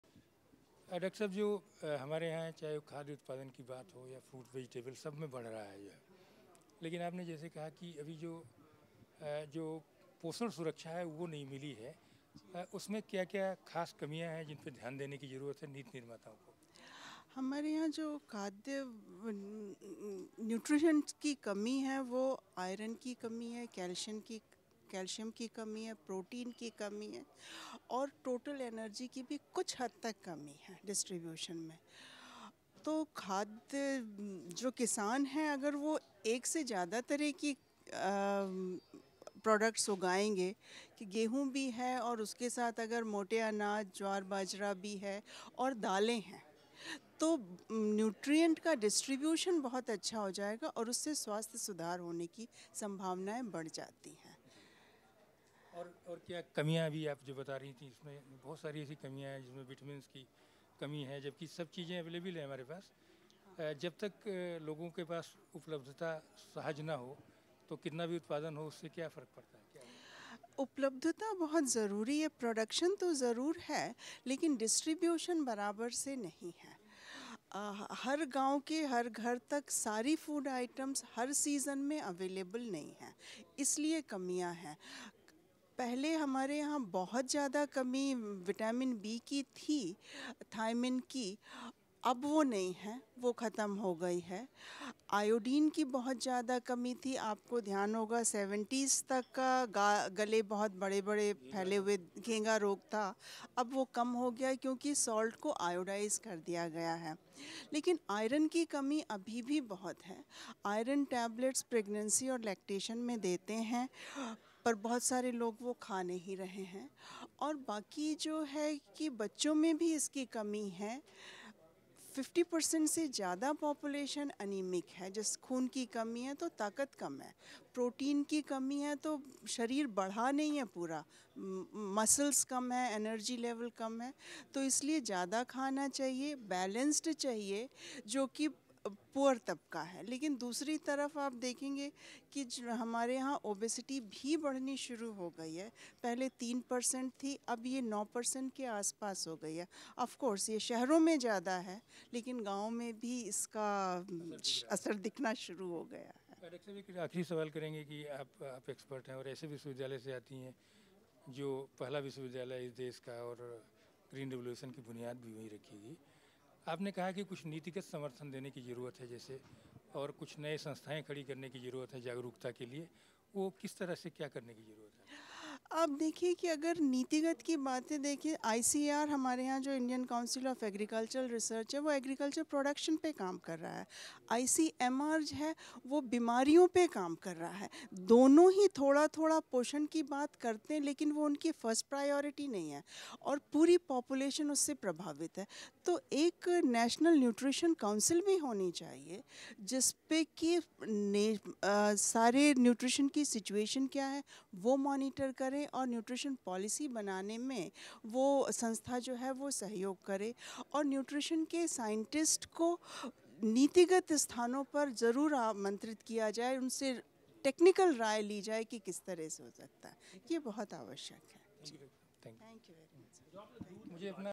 विख्यात वैज्ञानिक डॉ. रीता सिंह रघुवंशी से अरविंद कुमार सिंह की बातचीत
डा.रीता सिंह रघुवंशी, डीन कालेज आफ होम साइंस, गोविंद बल्लभ पंत कृषि विश्वविद्यालय, पंतनगर का मानना है कि हमारे यहां सबसे ज्यादा पौष्टिकता की कमी है। उसे दूर करने की दिशा में ठोस रणनीति की दरकार है।